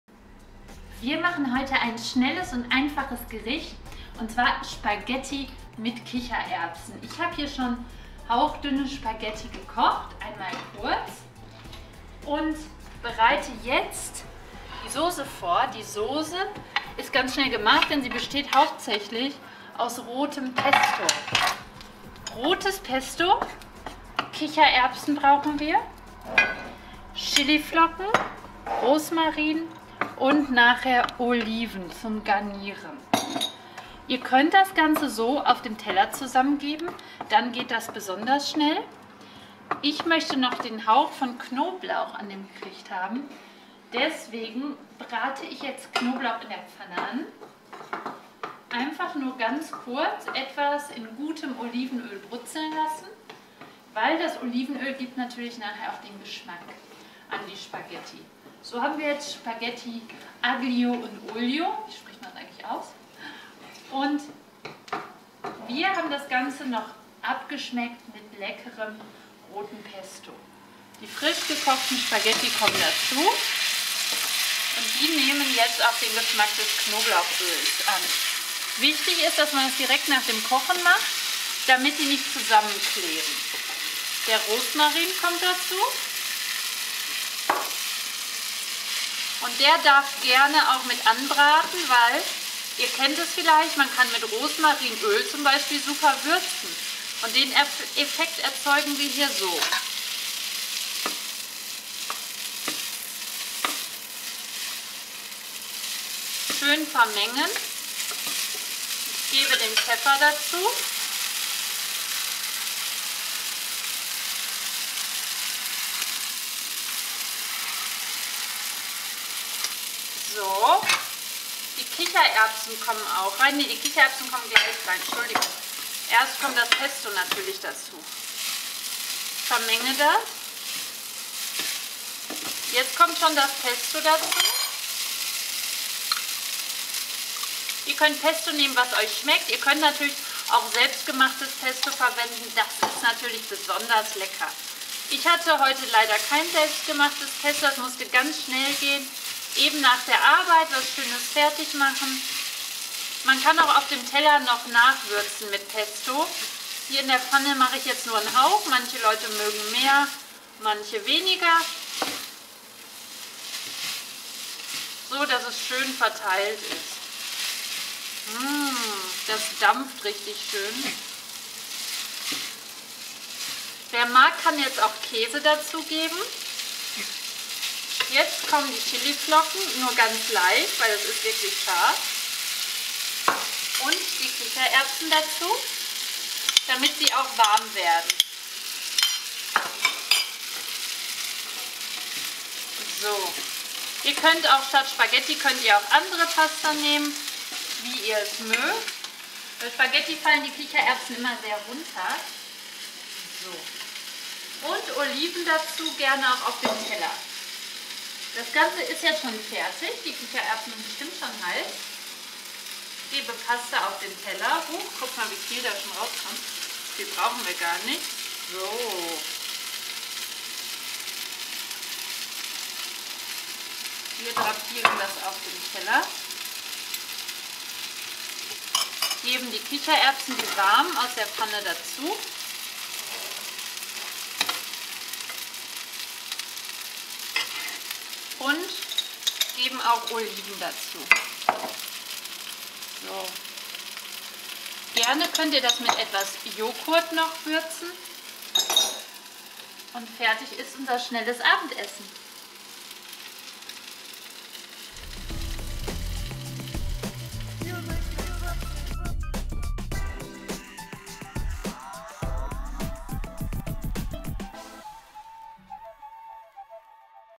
Pasta mit Pesto und Kichererbsen - vegetarisches Rezept von einfachKochen
Wir machen heute Pasta mit Pesto und Kichererbsen ein leckeres vegetarisches Rezept von einfachKochen.

Werdet Fan auf Facebook!

Kontakt im Impressum auf der Blog-Seite
Kanal: